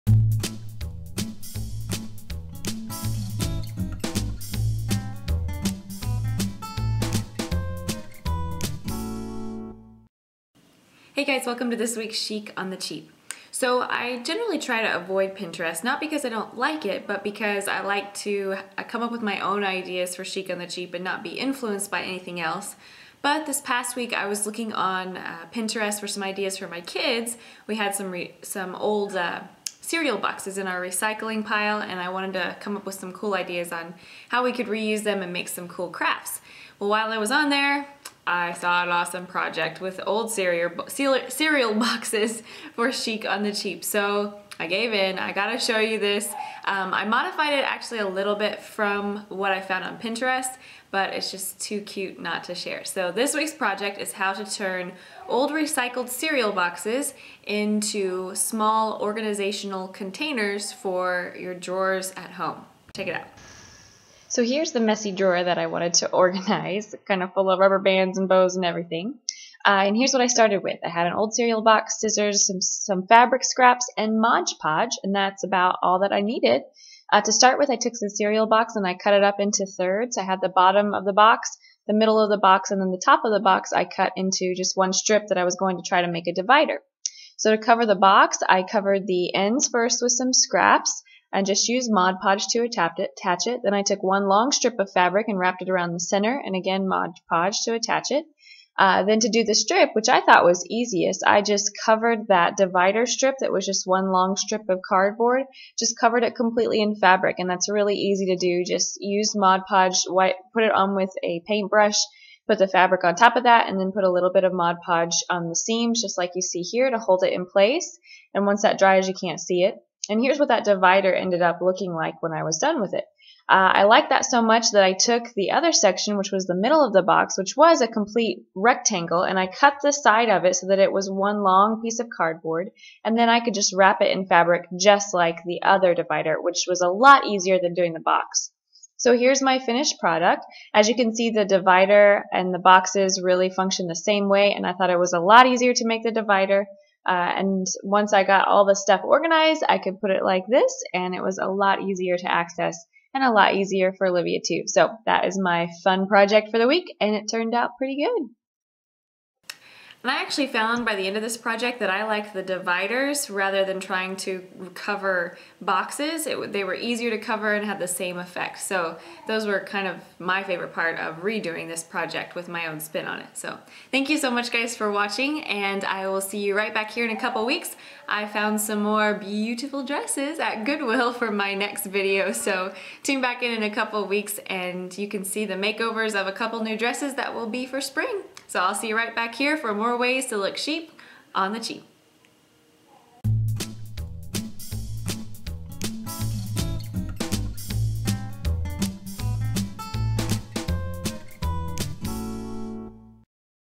Chic on the Cheap: Cereal Box Organizers
Turn an old cereal box into cute drawer dividers! An easy and chic way to get organized in just a few easy steps!